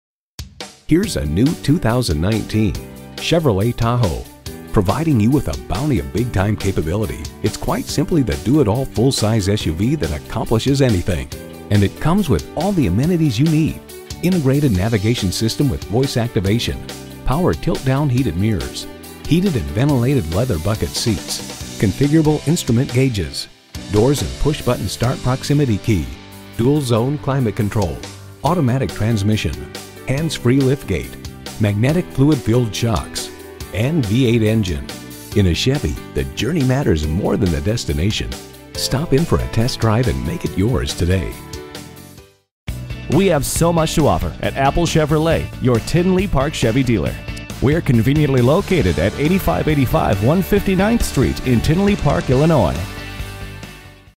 2019 Chevrolet Tahoe Tinley Park IL Orland Park, IL #19-0474
Year: 2019
Make: Chevrolet
Model: Tahoe
Trim: Premier
Engine: EcoTec3 5.3L V8
Transmission: 6-Speed Automatic
Color: Beige
Interior: Jet Black
Stock #: 19-0474
VIN: 1GNSKCKC9KR145510
Pearl 2019 Chevrolet Tahoe Premier 4WD 6-Speed Automatic EcoTec3 5.3L V8 4WD, Jet Black Leather, ABS brakes, Compass, EASY LOADING Power Liftgate, Electronic Stability Control, Fog Lamps, Front dual zone A/C, Front Heated Reclining Bucket Seats, GREAT OPTION WHEN THE ROADS ARE SLICK! TRACTION CONTROL!, Heated 2nd Row Seats, Heated AND VENTILATED FRONT SEATS, Heated door mirrors, Heated rear seats, Illuminated entry! Awesome safety feature!, Low tire pressure warning, Power-Adjustable Accelerator & Brake Pedals, Remote Keyless Entry, Remote keyless entry, Upgraded 18" POLISHED ALUMINUM WHEELS.brbrRecent Arrival!

Address:
8585 West 159th Street